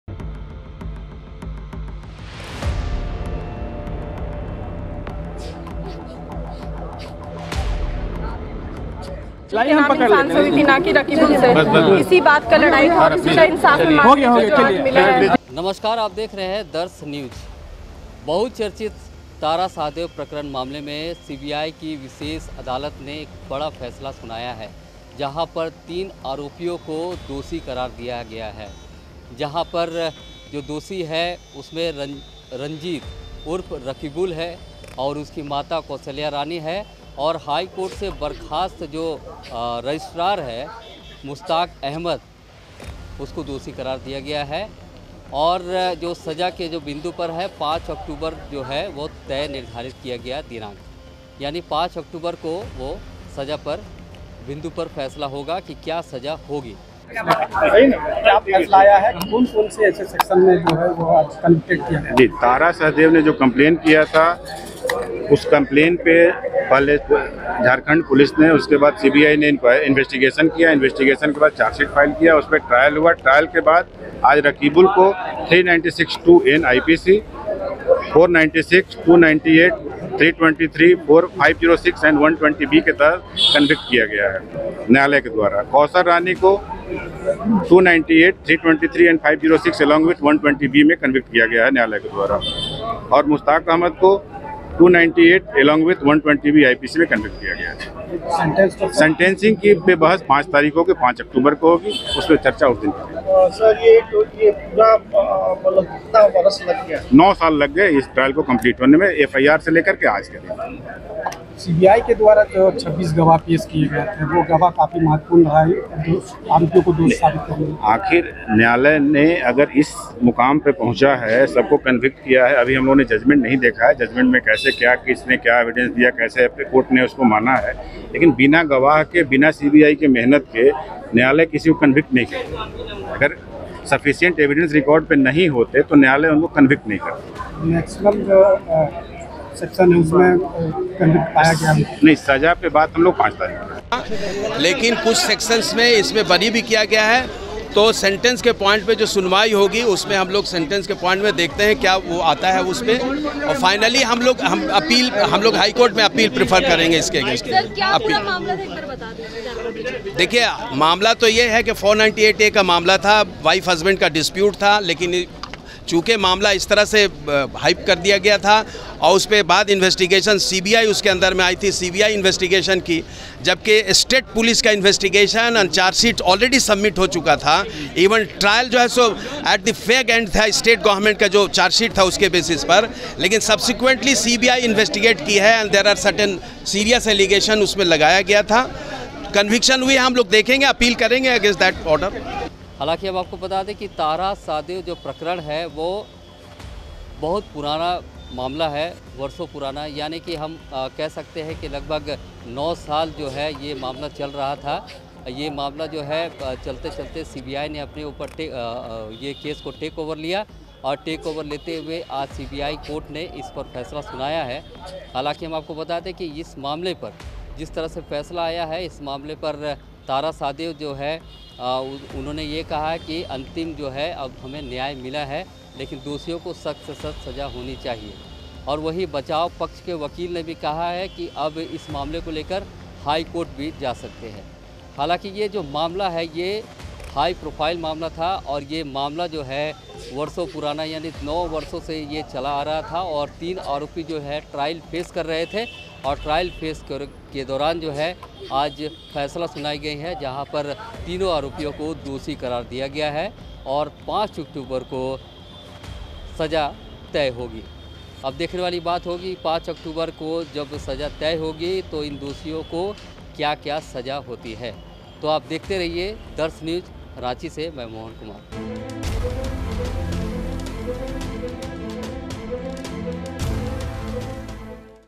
National Shooter Tara Shahdev मामले में CBI स्पेशल कोर्ट ने रंजीत समेत तीन को दोषी करार दिया
darshnewsviral darshnewsworld National Shooter Tara Shahdev मामले में CBI स्पेशल कोर्ट ने रंजीत समेत तीन को दोषी करार दिया